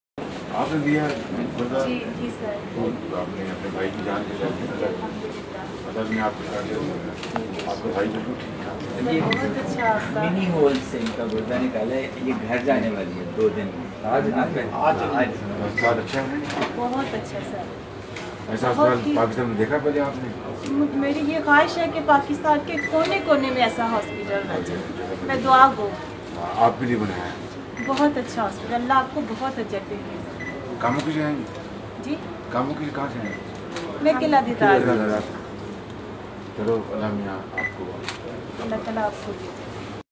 Proud of this daughter on donating kidney to his brother at PKLI
میاں محمد شہباز شریف: "مجھے دو باتوں پر بے حد فخر ہے، پنجاب حکومت کے بناۓ ہسپتال میں گردے کا پہلا ٹرانسپلانٹ کامیابی سے اور بالکل مفت ہوا اور دوسرا قوم کی اُس بیٹی پر جس نے اپنے بھائی کو گردہ کا عطیہ دیا۔ الحمد اللہ دونوں بچے صحت مند ہیں۔"